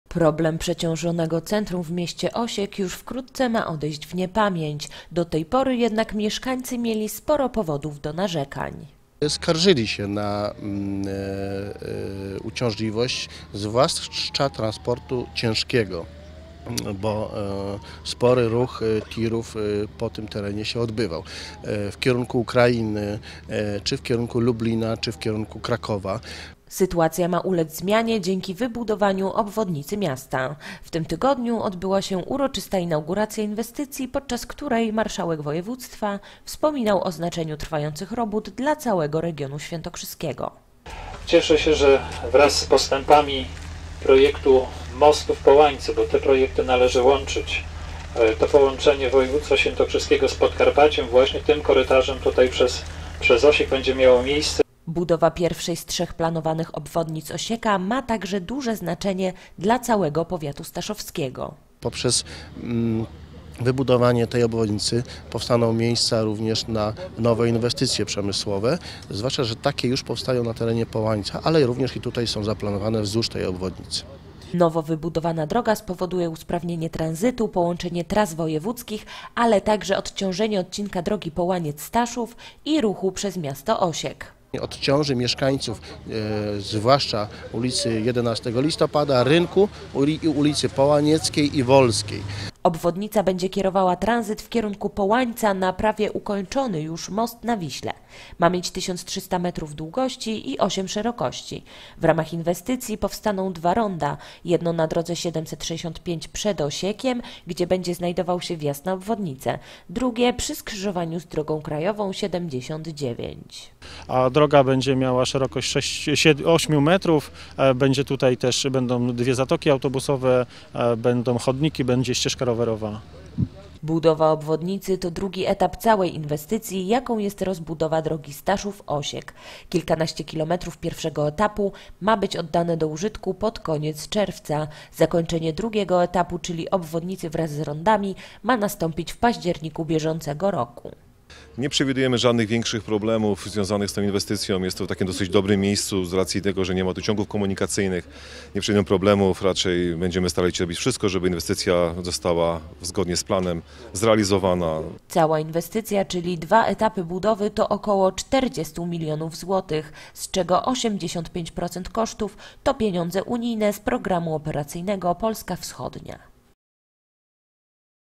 Łopaty uroczyście wbite. Ruszyła budowa obwodnicy Osieka
Na początku tego tygodnia ruszyła budowa jednej z trzech planowanych obwodnic miasta. Jej powstanie przyczyni się do wyprowadzenia ruchu tranzytowego z centrum miasta i tym samym zapewni spokój mieszkańcom.